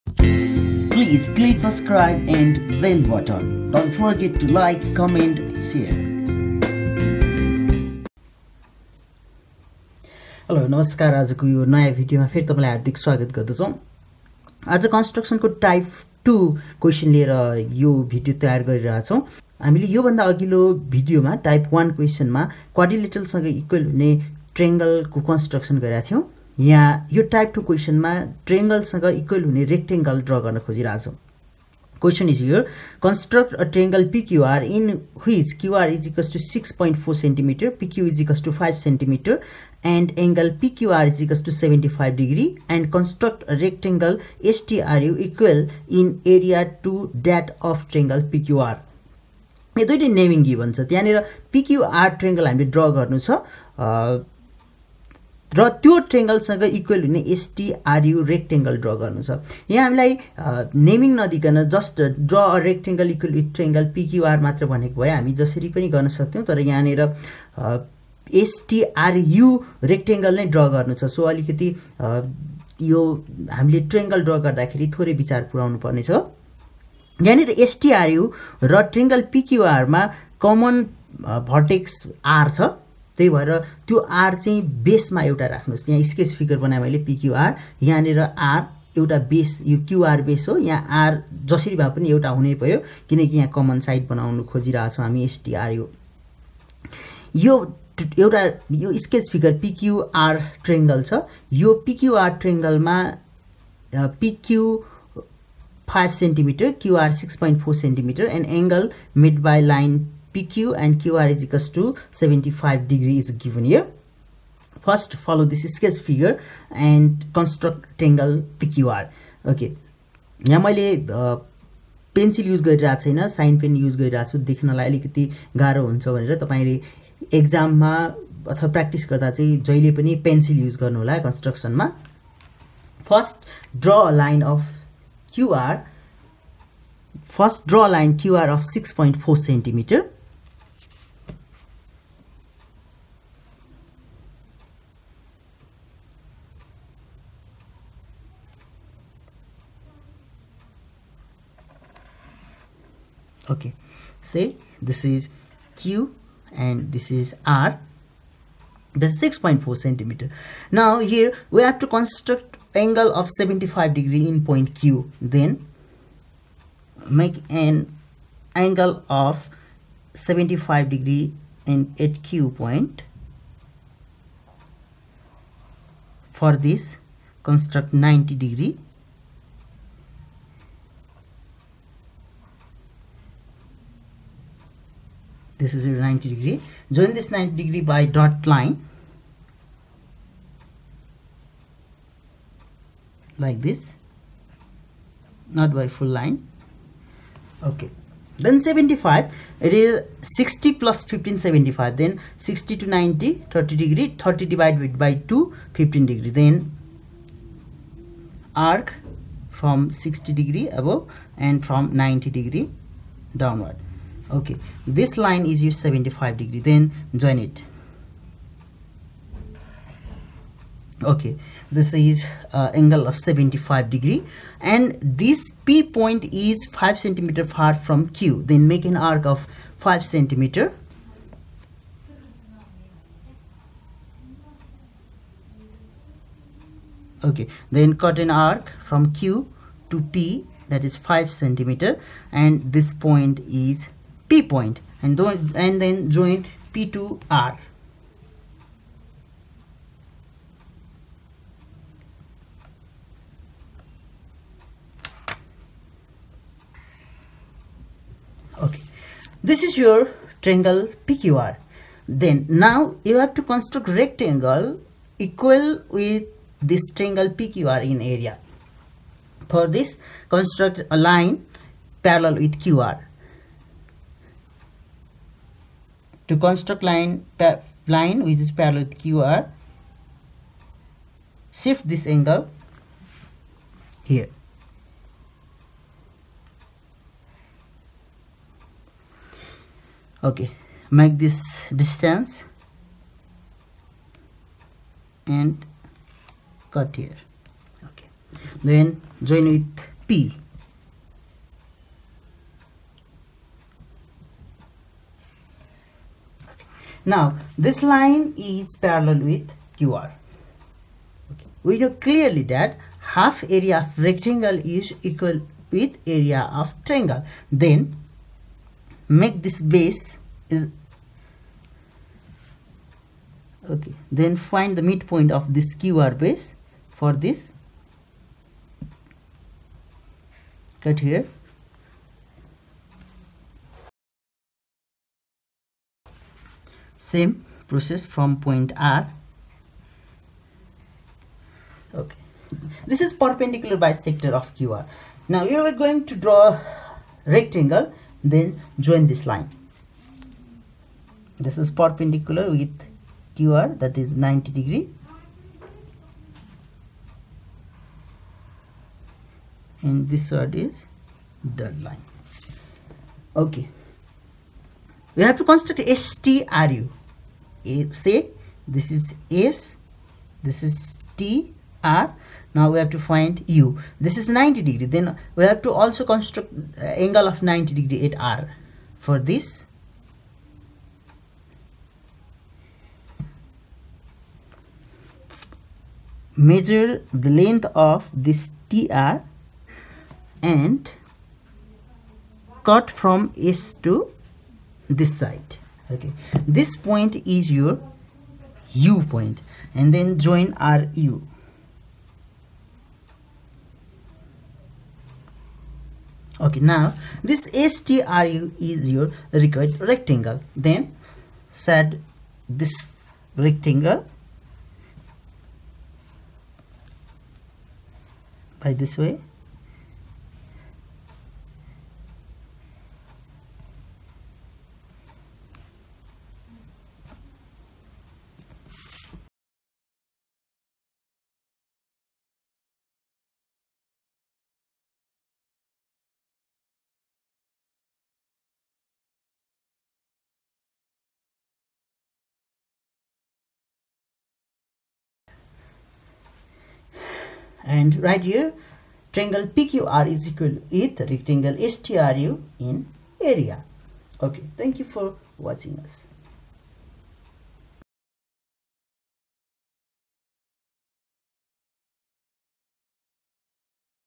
Construction Type 2: Area of Triangle is equal with Rectangle.
Construction: Area of Triangle is equal with Rectangle
Class 10 Construction: Rectangle and triangle.